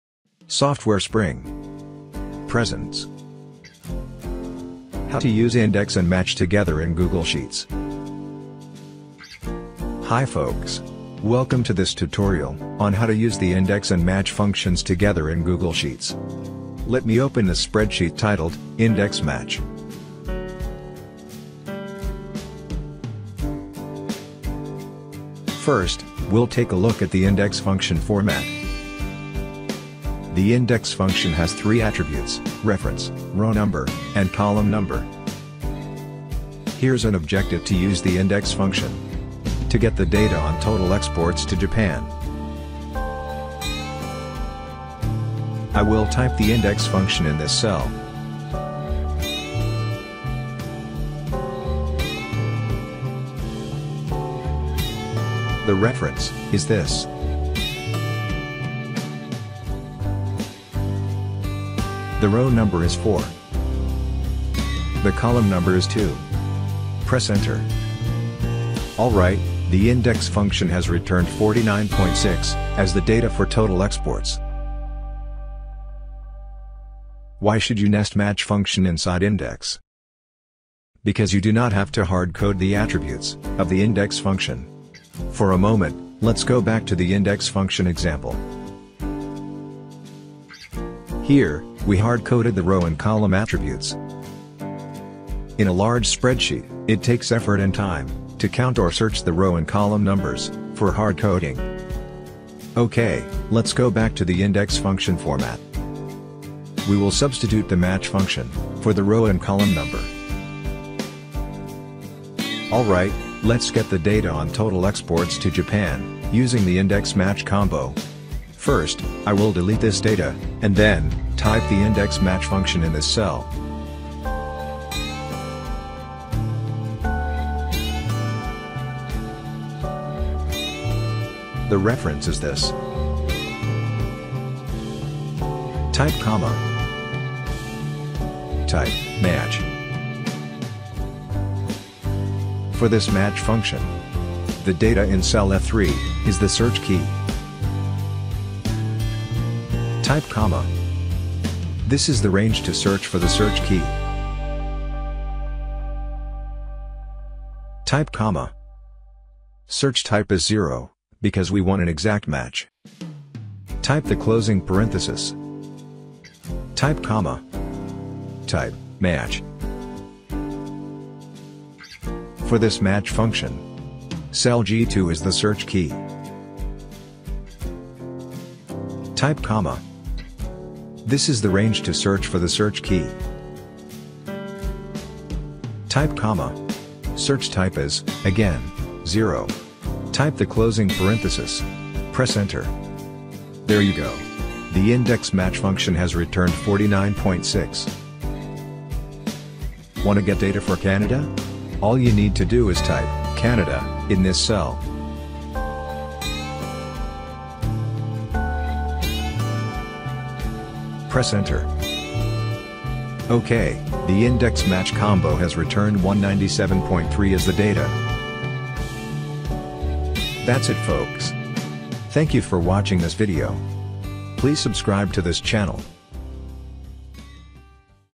Google Sheets INDEX | Google Sheets MATCH | How to Use INDEX and MATCH together
What is a reason to use the INDEX and MATCH functions together? If you do not wish to hard code the attributes of the INDEX function, then a solution is to nest the MATCH function inside the INDEX. A reason you would not want to hard code the attributes is when a Google Sheets spreadsheet is large.

In a large spreadsheet, it would be difficult to count the row and column number, because it takes effort, time, and may be error-prone. So, it would be a good idea to let the MATCH function do this job.

The match function returns a number. This is exactly what the INDEX function needs for its row and column attributes. Use the MATCH function twice inside the INDEX function, one for the row attribute and another for column.

Let's consider an example. Ref to the simple spreadsheet below:

Country Total Exports
Canada 197.3
Mexico 173.5
Japan 49.6

Say you want to extract the Total Exports data for Japan. The INDEX function for this task is:

=INDEX(reference, [row], [column])

=INDEX(A1:B4,4,2)

Because this is a very small spreadsheet, it's easy to hard code the INDEX function attributes. What if the spreadsheet was large and the data for Japan was in a row and column way down and across the spreadsheet? Further, what if you wanted the data for several other countries by simply typing the name of the country into a cell? In this case too, it would not be possible by using the INDEX function alone.

If you, however, nest the match function inside the INDEX function, you can extract data even if the spreadsheet is large, and get the data for another entity, for example, with reference to the spreadsheet above, for another country by simply typing its name. You can accomplish both these tasks, without hard coding the INDEX function attributes.

As an example, let's consider the above spreadsheet again and use the INDEX and MATCH functions together and extract the Total Exports data for Japan.

First, we'll take a look at the format of the MATCH function:

MATCH(search_key, range, [search_type])

Assume that cell E1 has the header, Country, and cell F1 has the header Total Exports. Further assume that cell E2 has the text Japan as the data.

For the row attribute of the INDEX function:

MATCH(E2, A1:A4, 0)

For the column attribute of the INDEX function:

MATCH(F1, A1:B1, 0)

Now, let's nest the above two MATCH functions inside INDEX:

=INDEX(A1:C4, MATCH(E2, A1:A4, 0), MATCH(F1, A1:B1, 0))

Type the above function in cell F2 and press Enter. The INDEX function will return 49.6 as the data for Total Exports for Japan.

Now, you can simply type another country's name in the spreadsheet in cell E2 and get the Total Exports data for it.

In conclusion, if you nest the MATCH function inside the INDEX function you do not have to hard code the INDEX function attributes. This method is especially helpful, as said before,
for large spreadsheets.